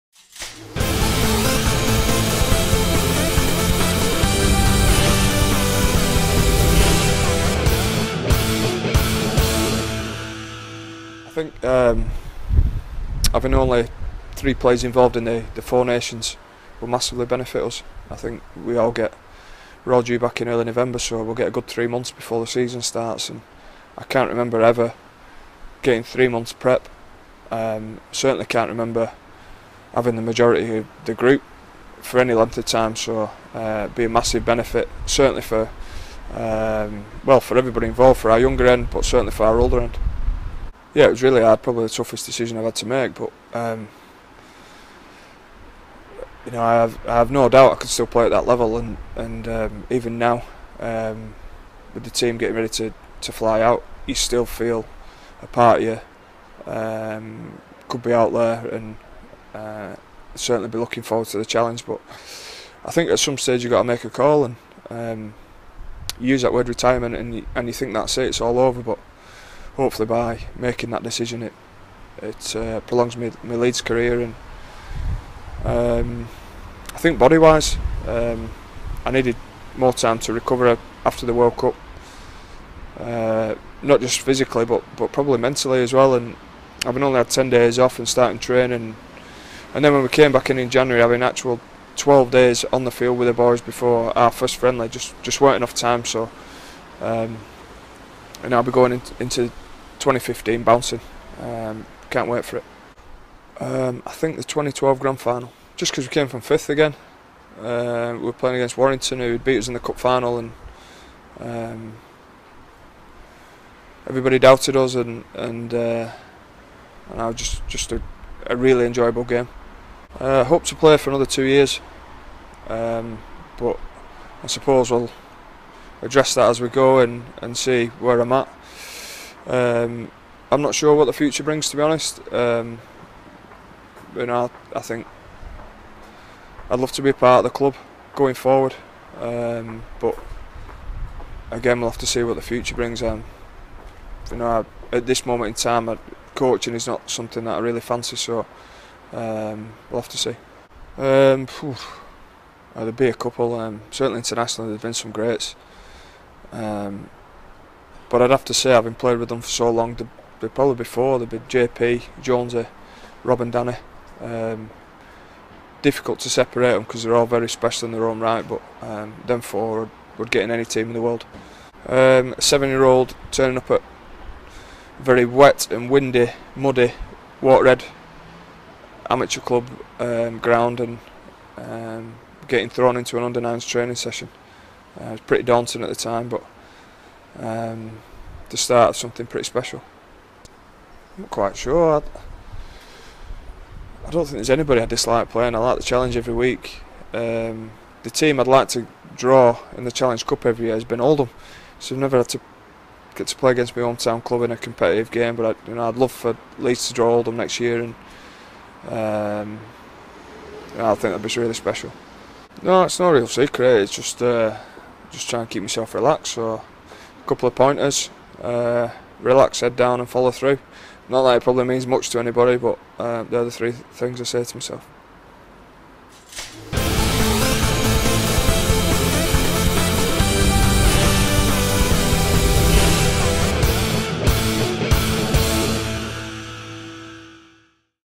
AskSirKev
We asked our Twitter followers to post their questions to Kevin Sinfield and the Rhinos skipper reflects on his England retirement, the best players he's played with and his favourite ever game amongst other great questions